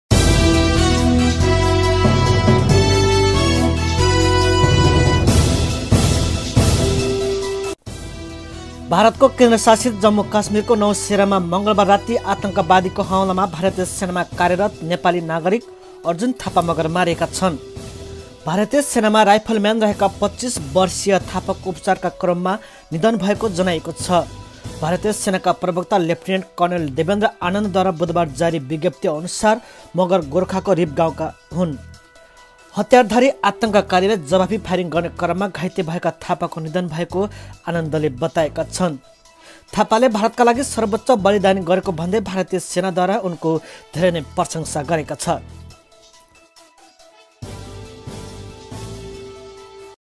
जम्मुकास्मिरमा भारतीय सेनामा कार्यरत नेपाली अर्जुन थापा मगर को मृत्‍यु
भारतको केन्द्र शासित जम्मु कास्मिरको नौशेरामा मंगलबार राति आतंकवादीको हमलामा भारतीय सेनामा कार्यरत नेपाली नागरिक अर्जुन थापामगर मारिएका छन्।
भारतीय सेनामा राइफलम्यान रहेका २५ वर्षीय थापाको उपचारका क्रममा निधन भएको जनाइएको छ। भारतीय सेनाका प्रवक्ता लेफ्टिनेन्ट कर्णेल देवेन्द्र आनन्दद्वारा बुधबार जारी विज्ञप्ति अनुसार मगर गोरखाको रिप गाउँका हुन्। हतियारधारी आतंककारीलाई जवाफी फायरिङ गर्ने क्रममा घाइते भएका थापाको निधन भएको आनन्दले बताएका छन्।

थापाले भारतका लागि सर्वोच्च बलिदानी गरेको भन्दै भारतीय सेनाले प्रशंसा गरेको छ।